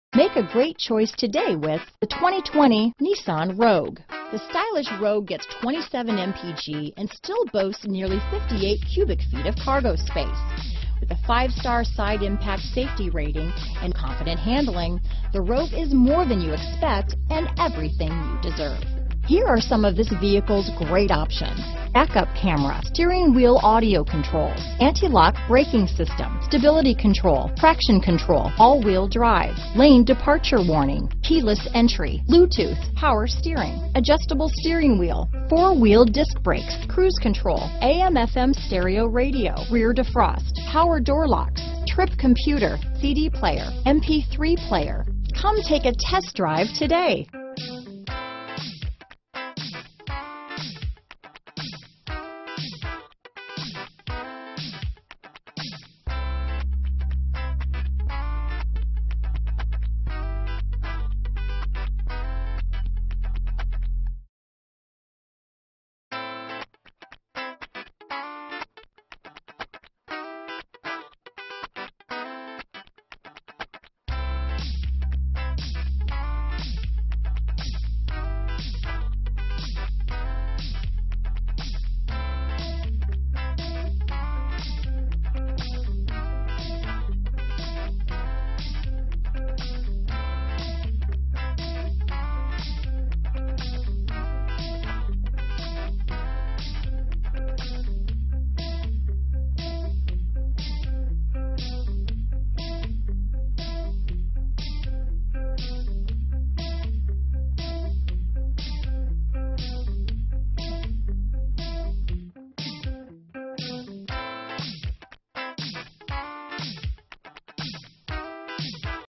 2020 Nissan Rogue Reno, Carson City, Northern Nevada, Roseville, Folsom, NV A7622
Gun Metallic New 2020 Nissan Rogue available in Reno, Nevada at Nissan of Reno. Servicing the Carson City, Northern Nevada, Roseville, Folsom, NV area.